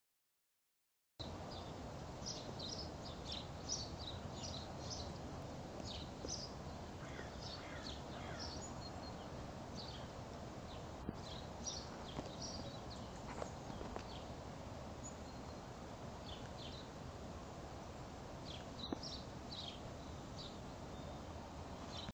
REFLEJARTE. 2012-13. Espacios emocionantes. F. M. Botín
Sergio Blanco. ALEGRÍA. "Por que me relaja mucho oír el canto de los pájaros y me produce bienestar, saber que por esa zona no pasan coches"